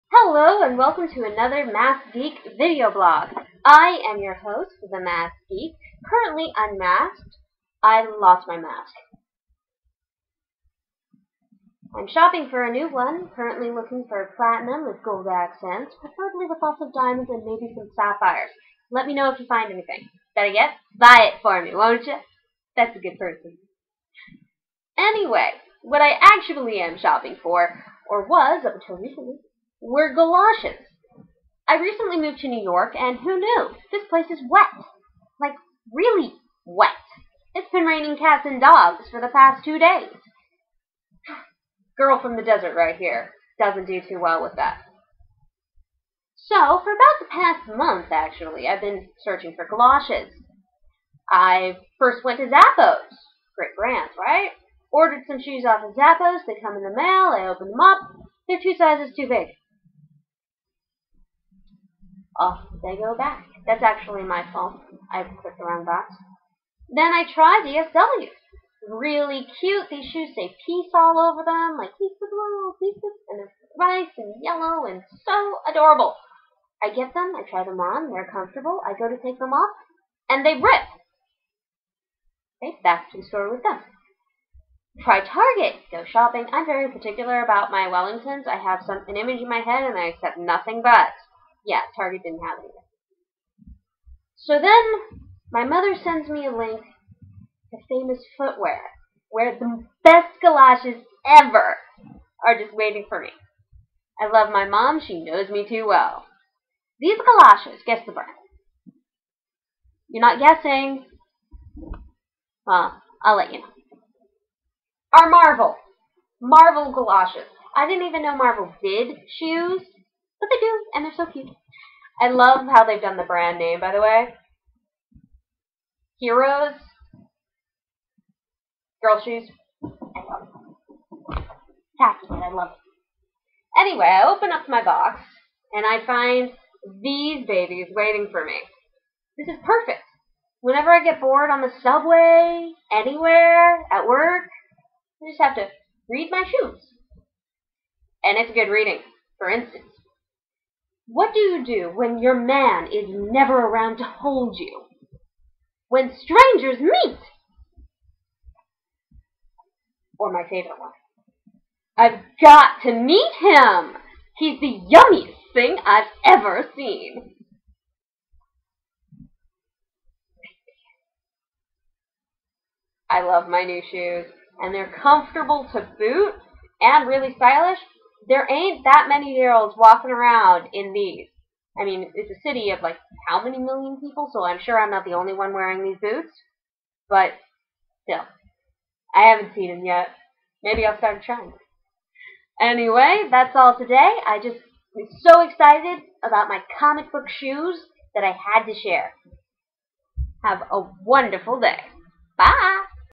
My new rain boots are geektastic!
I describe my hunt for the perfect rain boots that ends with something for the comic-book loving girl! I really didn't know this brand did rain boots. Best part? I can read my shoes!